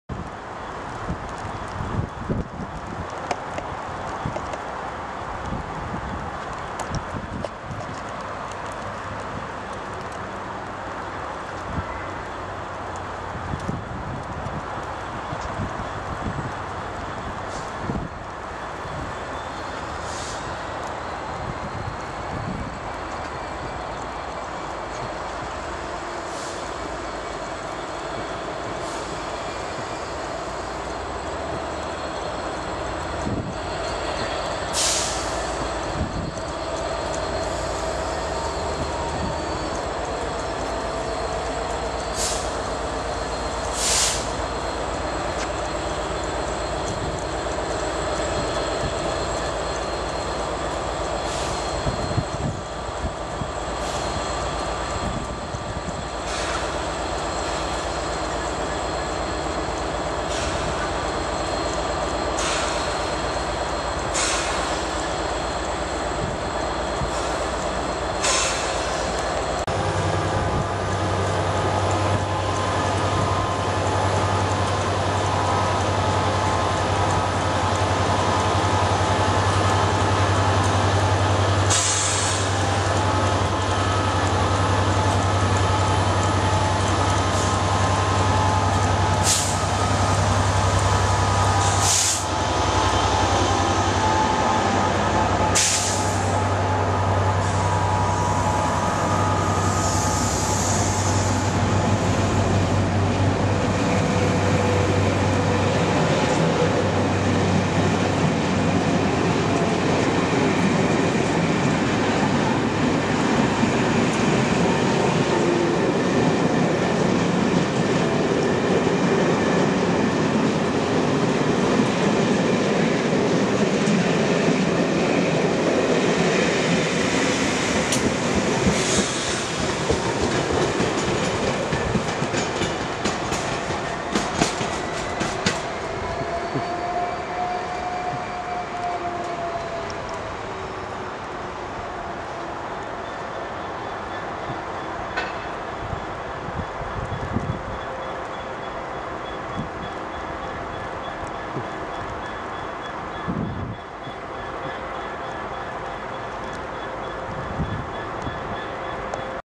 NORTHERN BELLA 1Z25 CHESTER TO SWINDON
CLASS 67003 & 67OO6 PASSING BESCOT ON A NORTHERN BELLA SERVICE